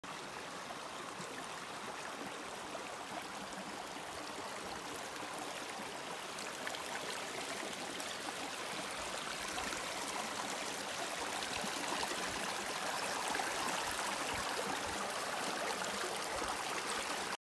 A typical Stream (bäck) in Northern Sweden
27 October 2020. As a part of my travel for work with Skogsstyrelsen